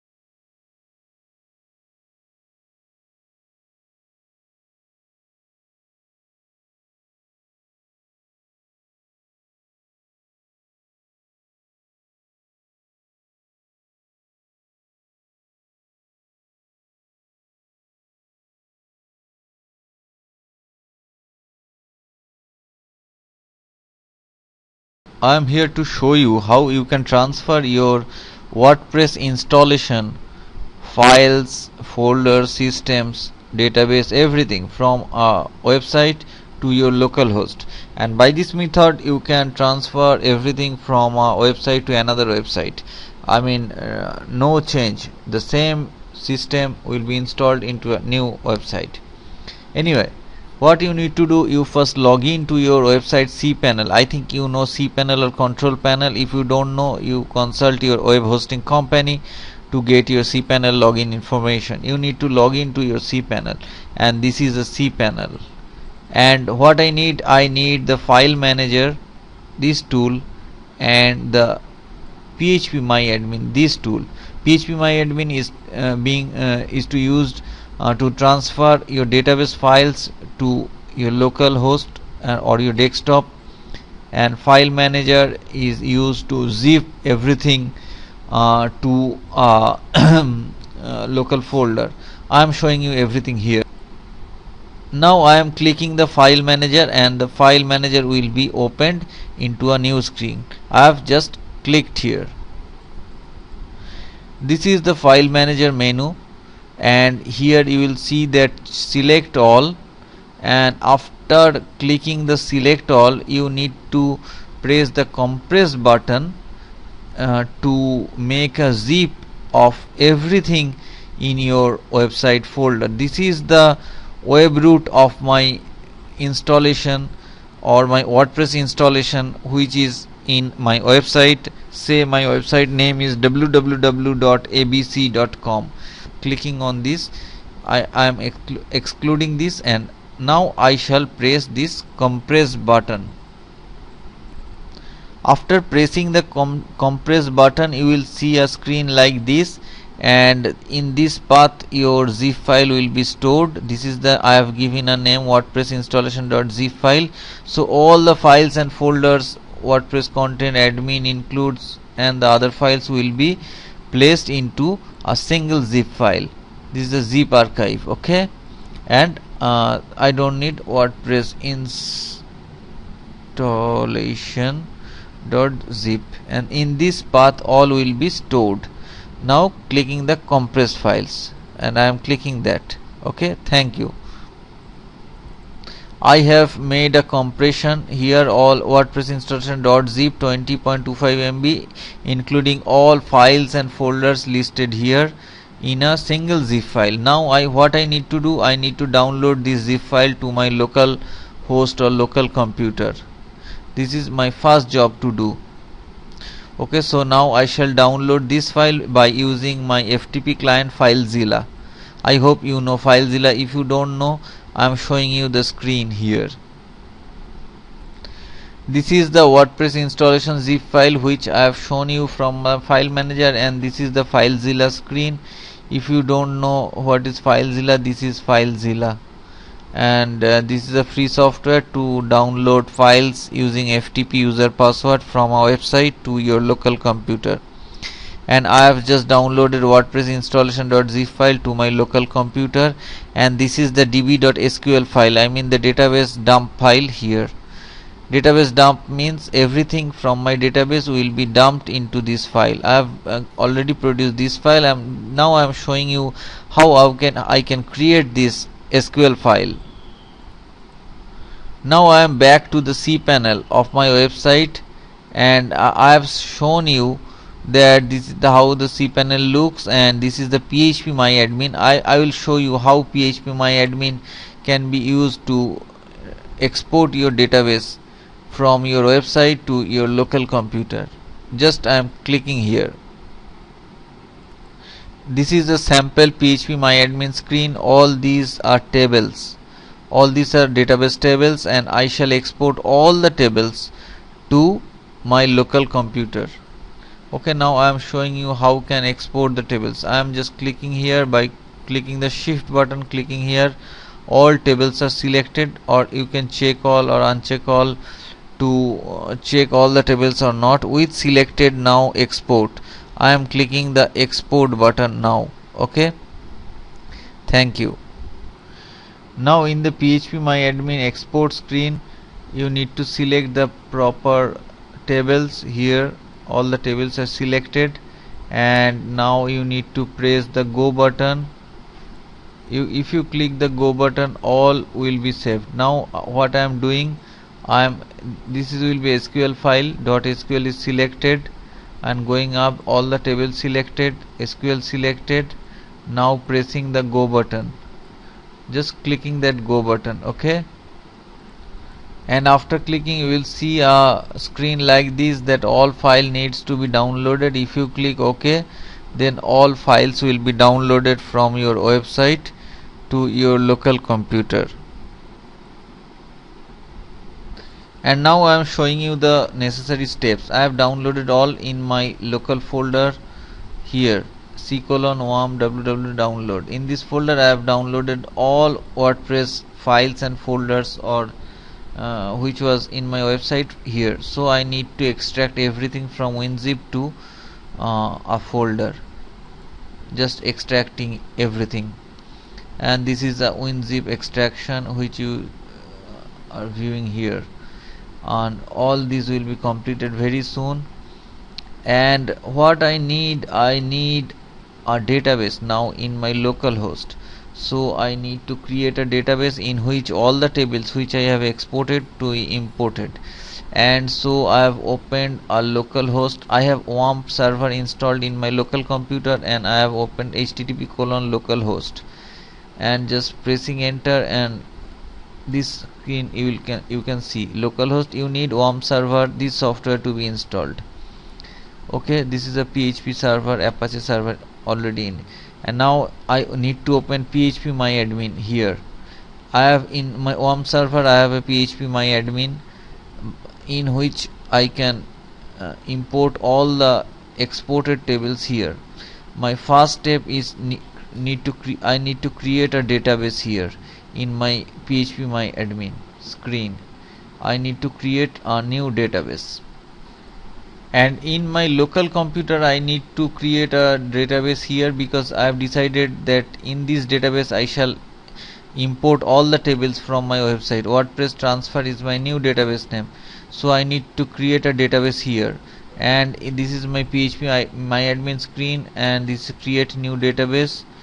How to transfer wordpress to new domain, Wordpress site migration Part 1
How to transfer wordpress to new domain : Part 1
After viewing this see this video PART 2:

Wordpress site migration:

Here I have shown how to transfer wordpress installation from a website " to localhost installation URL e.g "

This video tutorial on wordpress also shows you
* How to transfer WordPress Installation from one server to another server
* Changing domain of a wordpress site
* How to change web hosting company of a live wordpress site ________________________________________________

The Web Design Ecommerce Web Hosting Solutions.
Graphics Logo Multimedia Photoshop Flash Macromedia Director Specialists available.